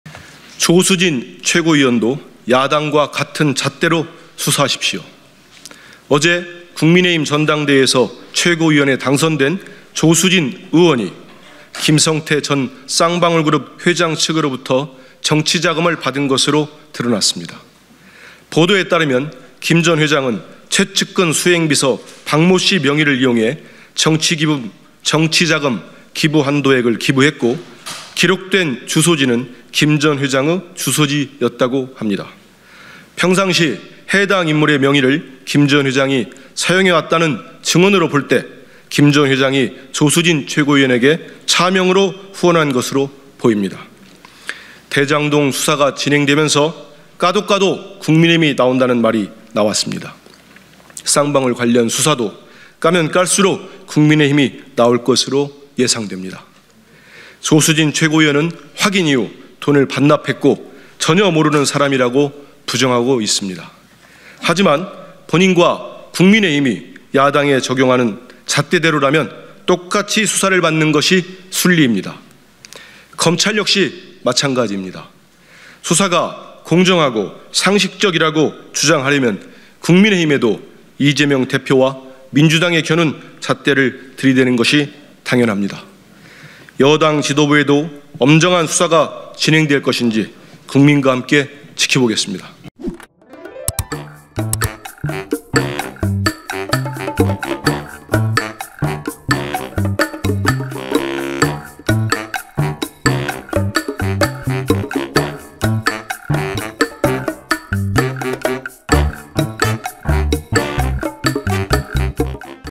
민주당 한민수 "조수진에 차명 후원한 김성태... 야당과 같은 잣대로 수사해!”
민주당 한민수 대변인이 23년 3월 9일 국회 소통관 기자회견장에서 브리핑을 하고 있다.